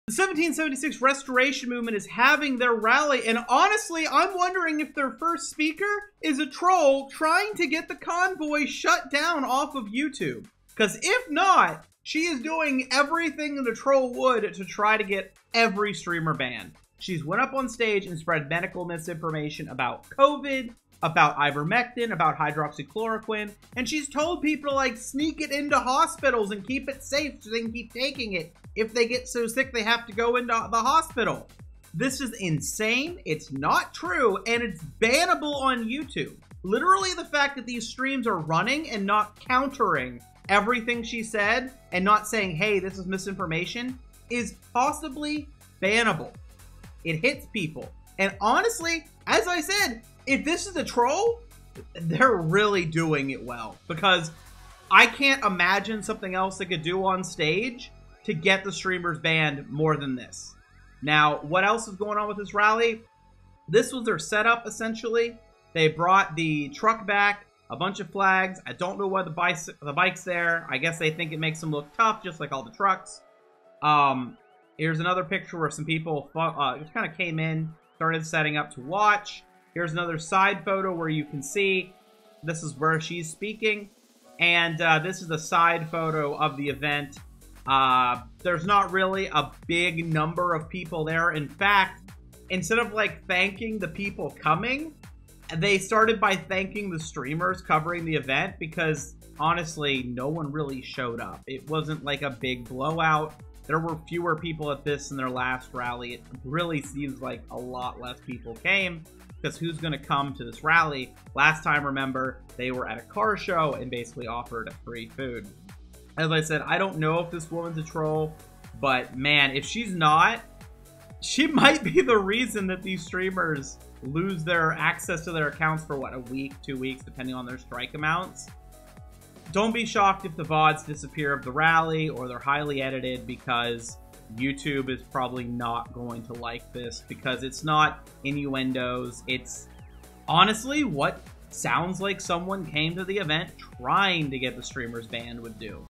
Medical Misinformation And Bad Turn Out At The 1776 Restoration Movement Rally
The 1776 Restoration Movement has terrible turn out at their rally and the only thing they have a lot of there is medical misinformation. The woman speaking could very easily be a troll because the level of her misinformation is exactly what a troll would do to try to cause bans for the convoy streamers.